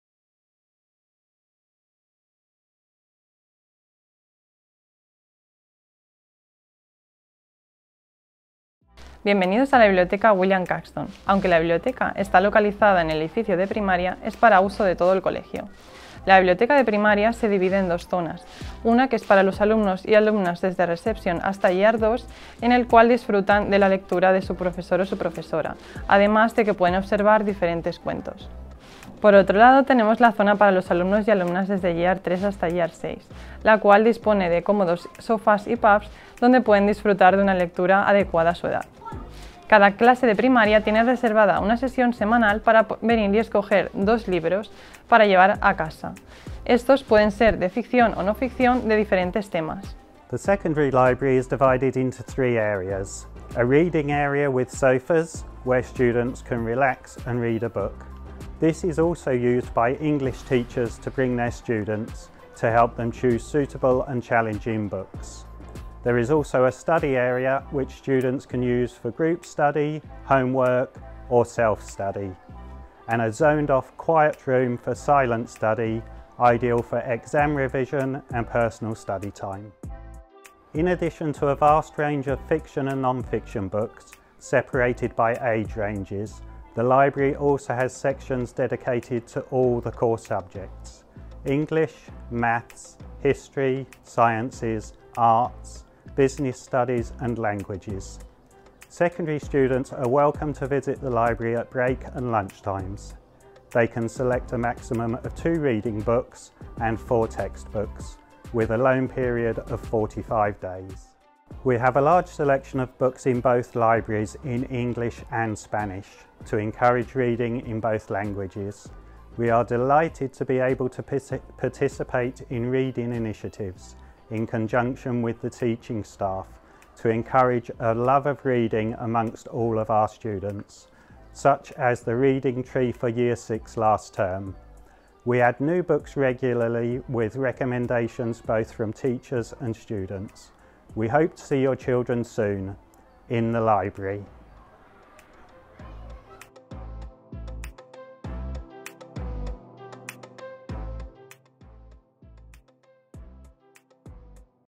Monthly Highlight: Do you know how William Caxton library works?
Video informativo sobre las características de la biblioteca William Caxton.

Informative video about the characteristics of the William Caxton Library.